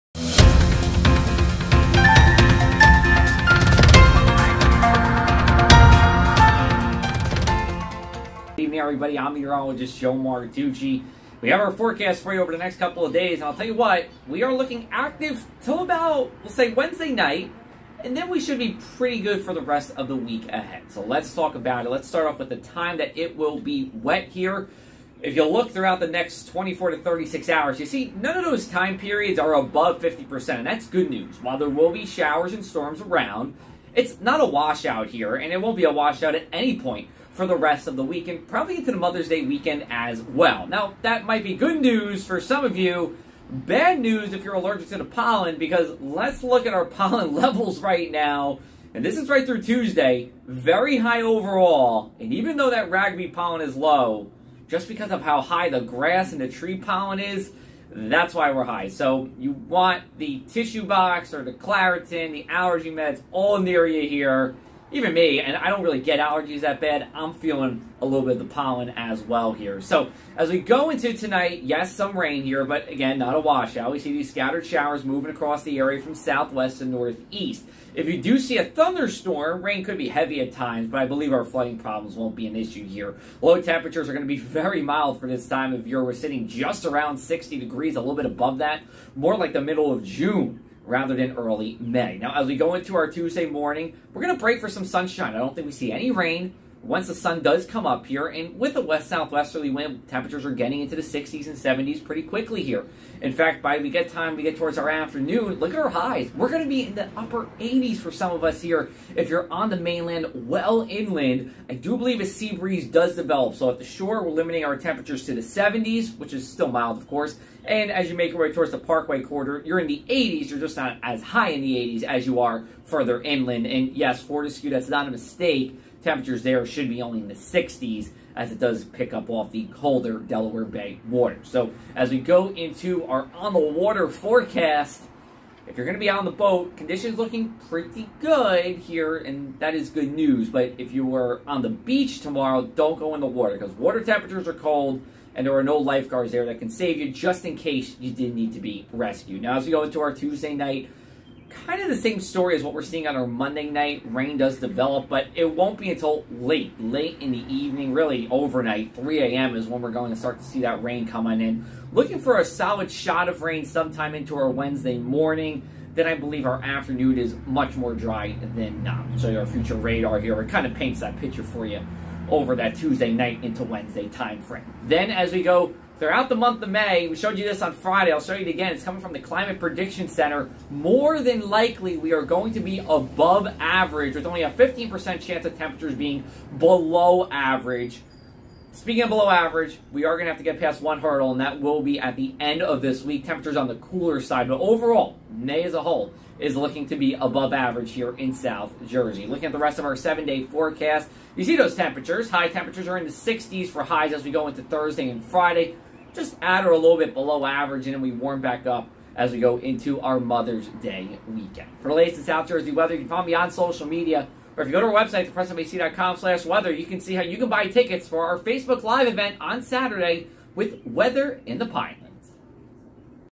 Your May 4 South Jersey forecast
Meteorologist Joe Martucci is on his deck for the Monday night forecast. Joe says that you'll need to stay under the overhang for parts of the night, and right through Wednesday. However, it won't be a washout and summery air will make an appearance. Joe has the details.